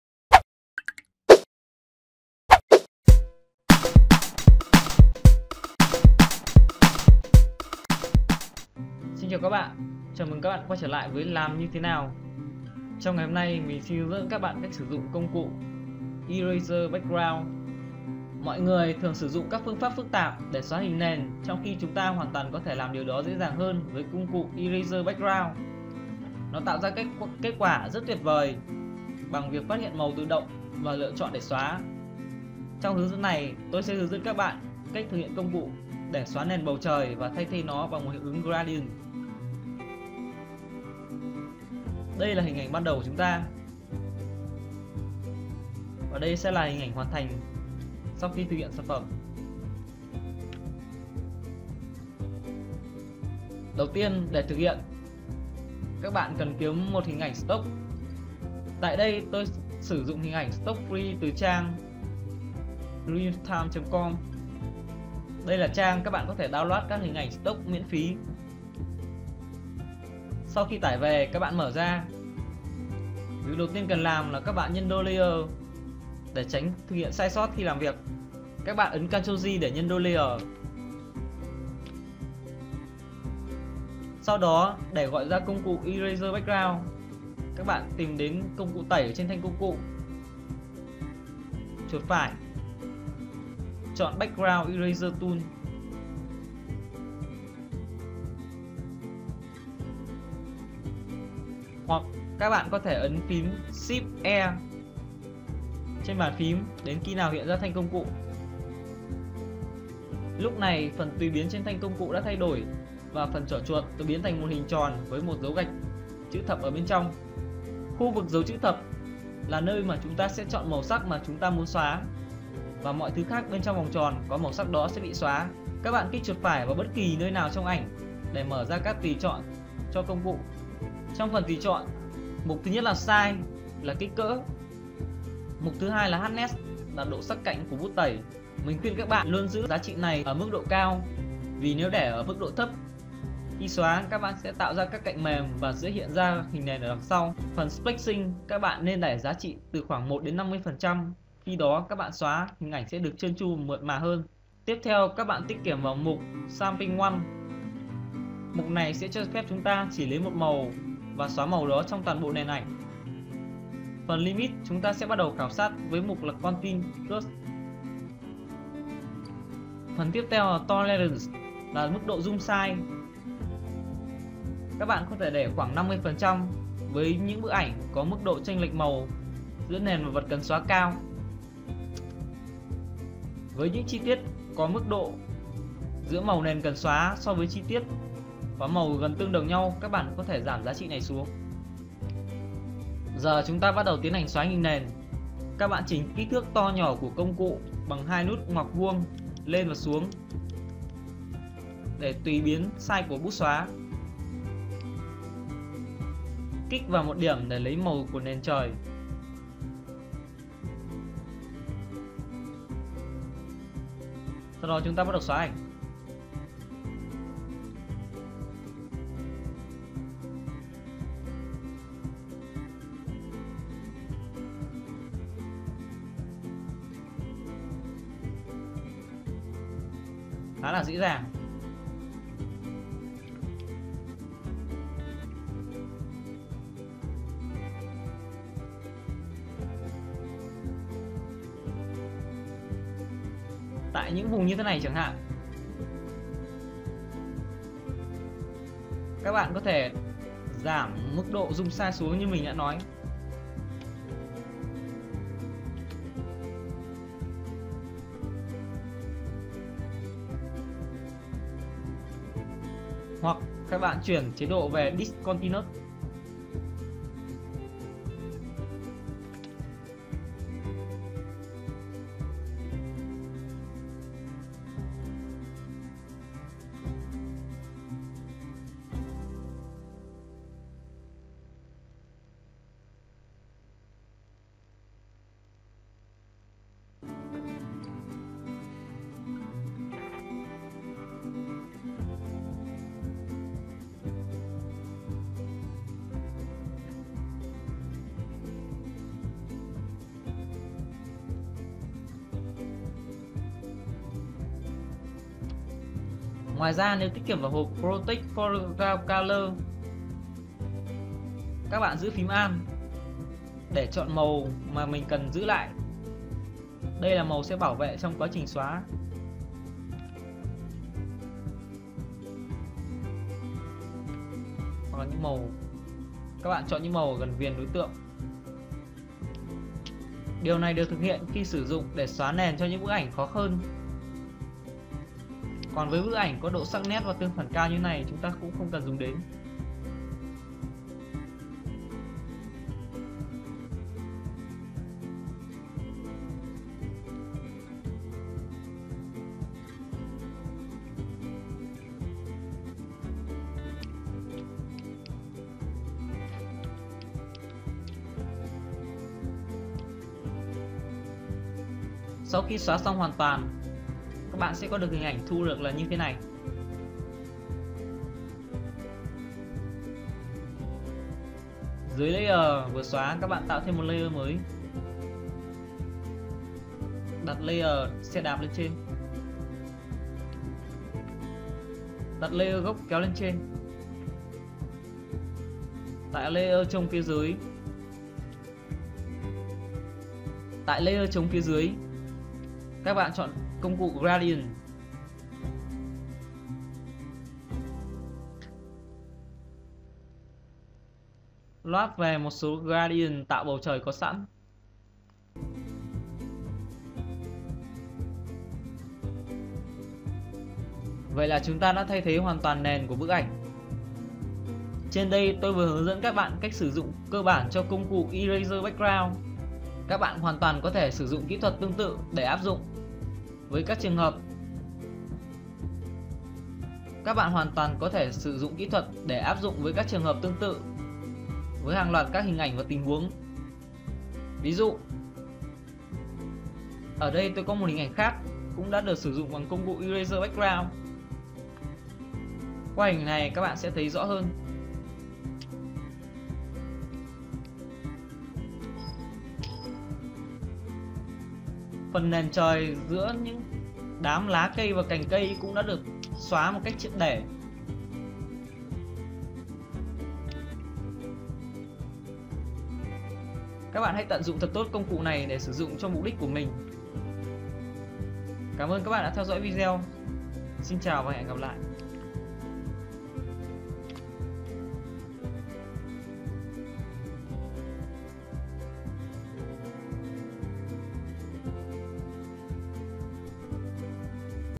Cách xóa nền ảnh trong Photoshop sử dụng công cụ Eraser Background
Hướng dẫn cách xóa nền nhanh trong Photoshop sử dụng công cụ Eraser Background